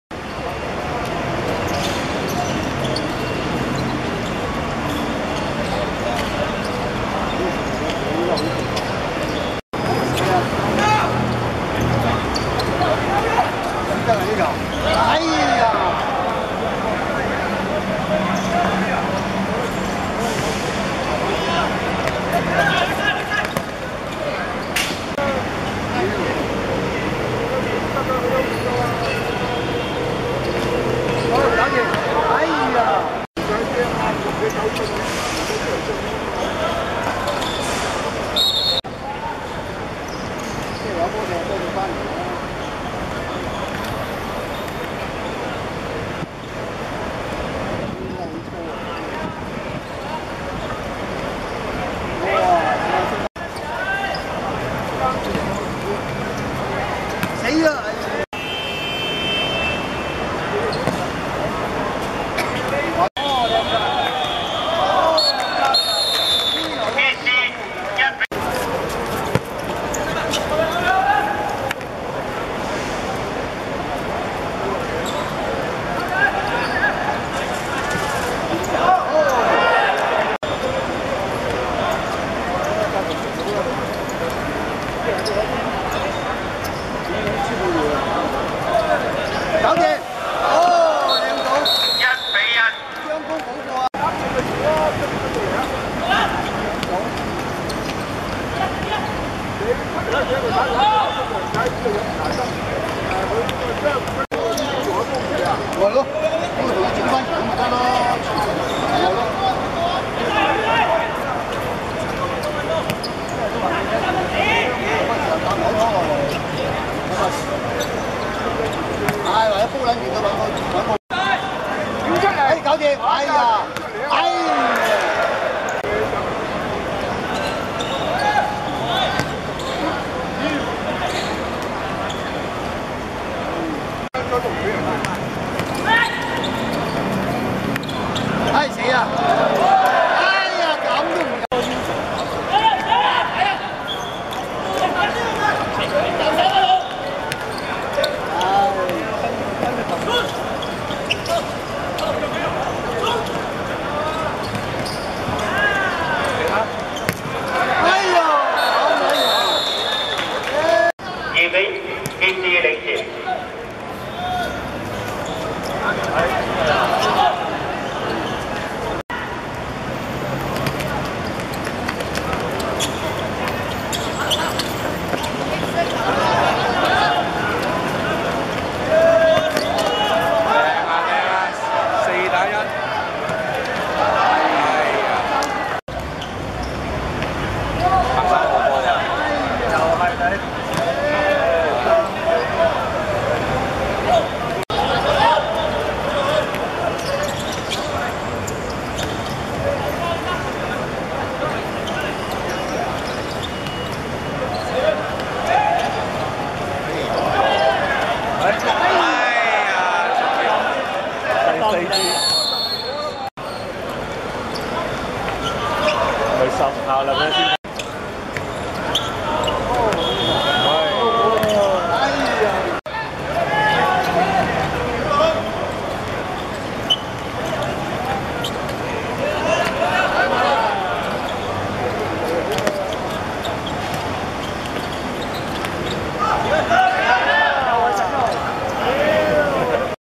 傑志vs栢禧(2013.7.3.東南海盃少年組準決賽)精華
2013.7.3.旺角麥花臣球塲 16:20 傑志(藍) 2:1 栢禧(白)